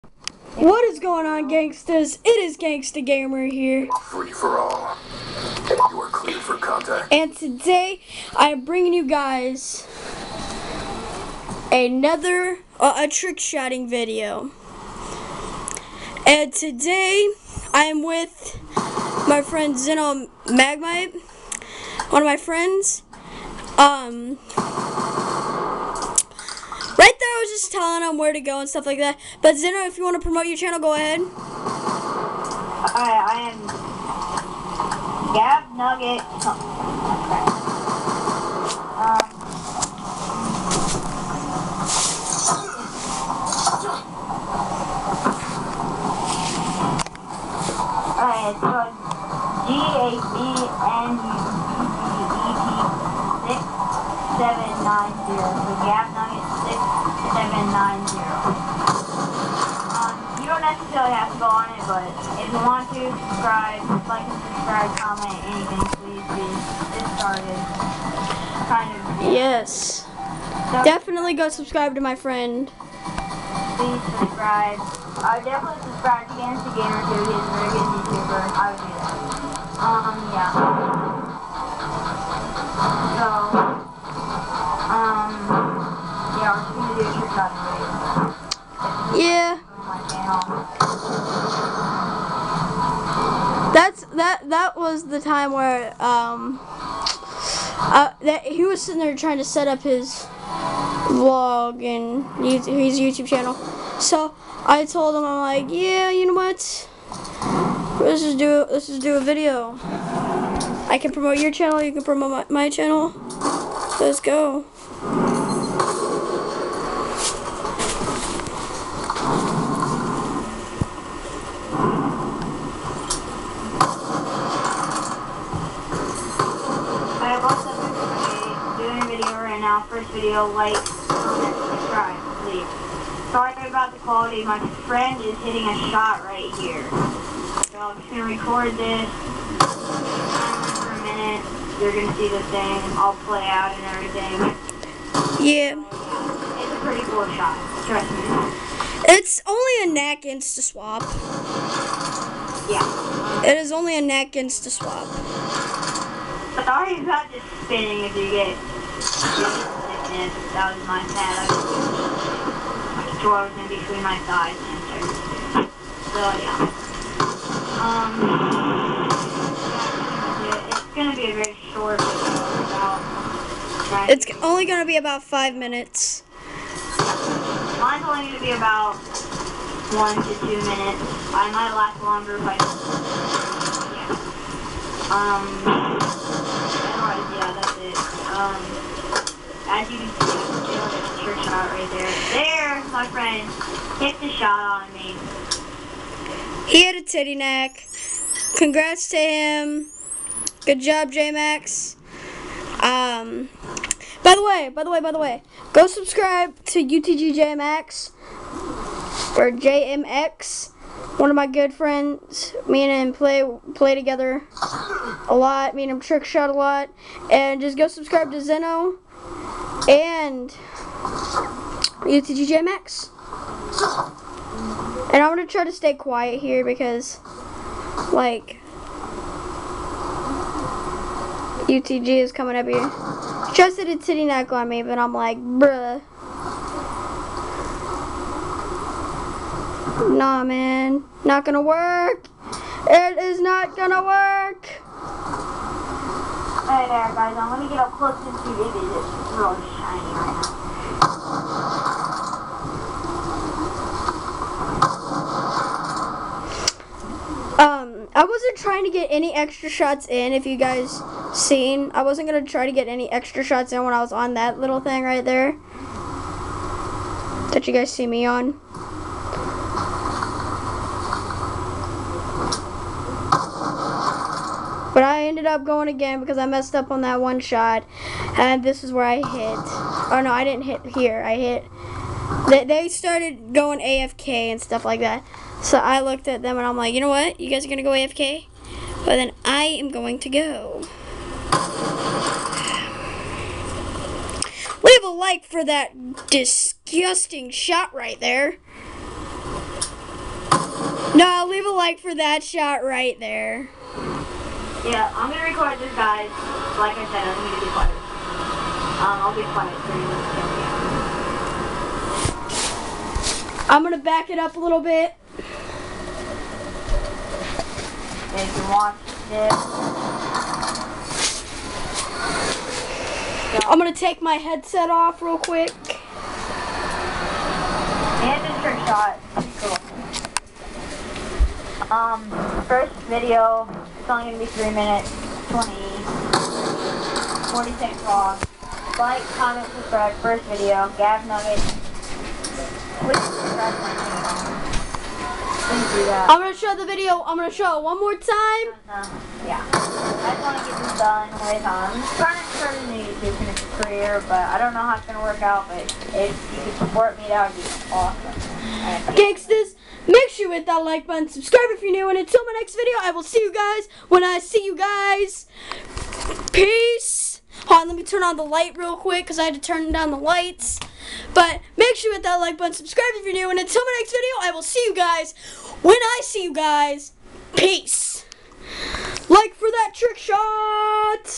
trickshotting with friends
sorry for the bad quality i was recording by hand  because i don't have a tripod yet so enjoy the video.